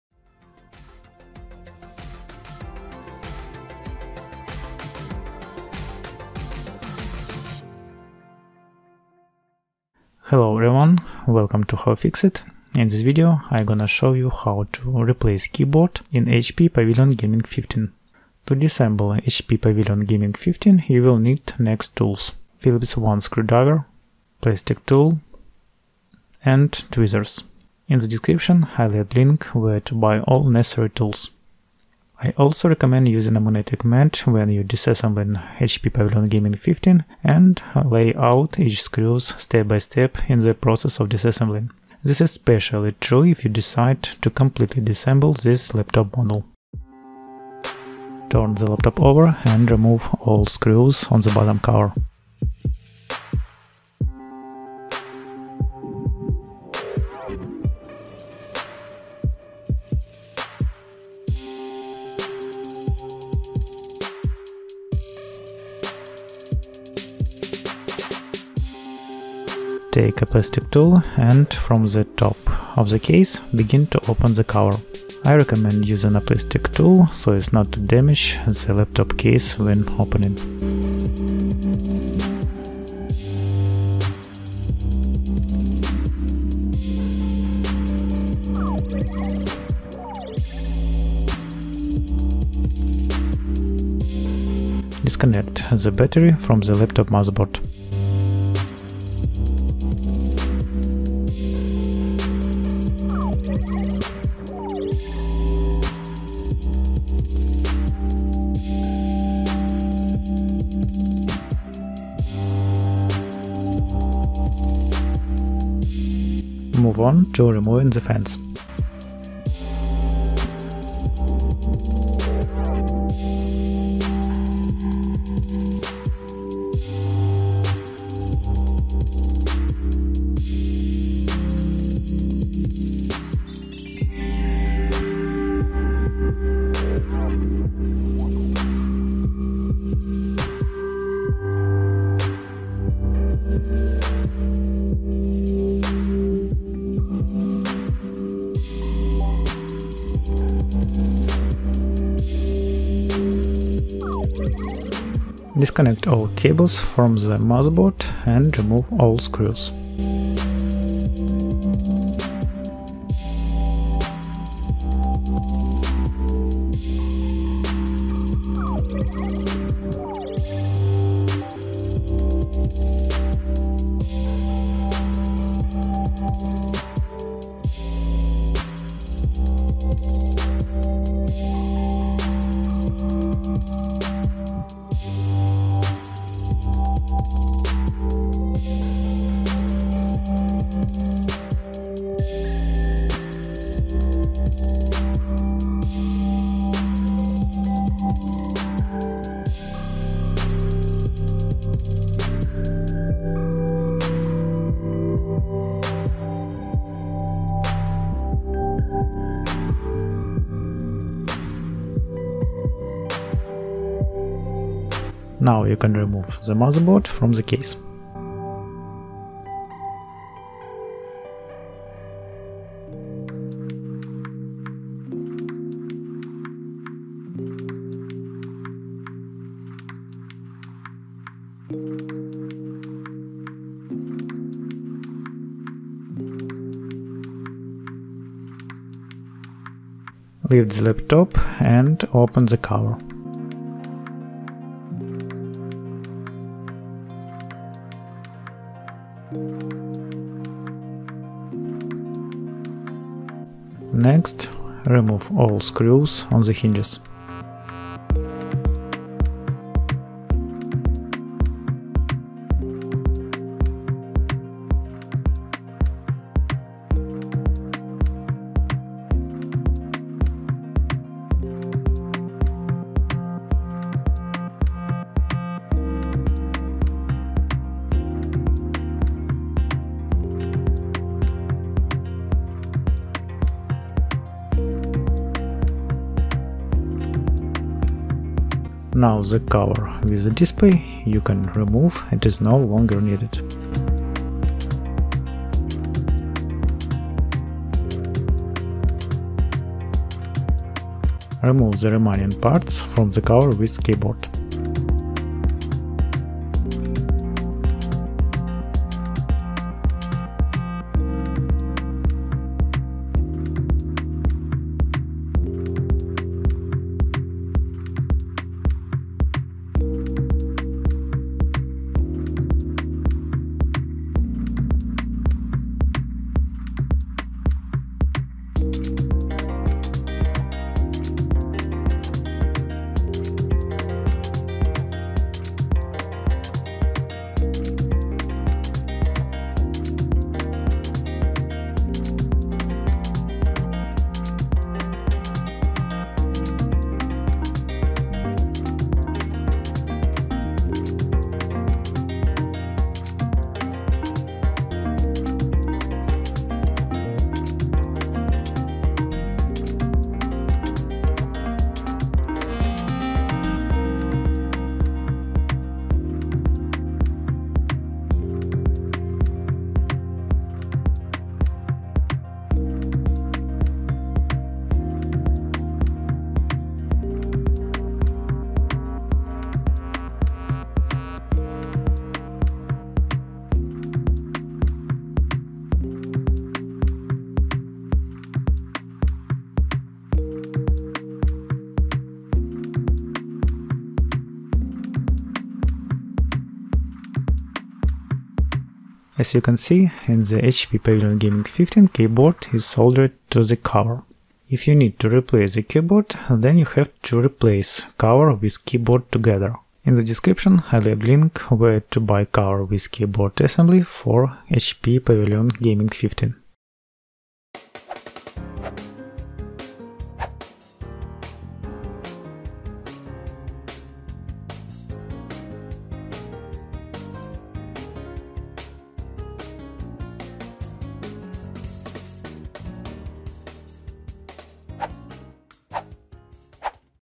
HP Pavilion Gaming 15 Keyboard Replacement | Fix Broken/Not Working Keyboard
Learn step-by-step how to replace unresponsive keyboard assembly in your HP Pavilion Gaming 15-cx laptop quickly and safely.

*DESCRIPTION*
Is your HP Pavilion Gaming 15 laptop keyboard malfunctioning or damaged? In this detailed step-by-step video guide by HowFixit, you'll learn exactly how to replace your keyboard safely and efficiently. We'll cover every necessary step, from removing screws to disconnecting cables, ensuring you avoid common mistakes during disassembly.

🌟 *Important Note:*
The keyboard on the HP Pavilion Gaming 15 is soldered to the laptop's top cover. Therefore, replacing the keyboard involves replacing the entire top cover assembly.

Whether you're dealing with stuck keys, liquid spills, or just worn-out keys, this video will save you time and help restore your laptop’s functionality. Be sure to follow each step carefully and safely. For your convenience, timestamps are provided below for quick navigation.

⏲️ Time Codes
00:00 - Introduction & Tools Needed
00:47 - Removing Bottom Cover Screws
01:10 - Safely Opening the Laptop Case
01:34 - Disconnecting Laptop Battery
01:44 - Removing Fans and Disconnecting Cables
03:35 - Motherboard Removal Process
04:00 - Removing Display Assembly
06:36 - Keyboard Replacement Explanation

Question: Can I replace individual keys on the HP Pavilion Gaming 15 keyboard?
Answer: No, keys are soldered, requiring replacement of the entire keyboard assembly.

Question: Where can I buy the keyboard assembly for HP Pavilion Gaming 15?
Answer: Links are provided in the video description for genuine parts.

Question: Do I need thermal grease for keyboard replacement?
Answer: Thermal grease is useful for CPU/GPU maintenance during laptop disassembly.

Question: Is it safe to replace the keyboard myself?
Answer: Yes, with careful attention to instructions and recommended tools.

Question: Why use a magnetic mat during repair?
Answer: It helps keep screws organized and prevents loss.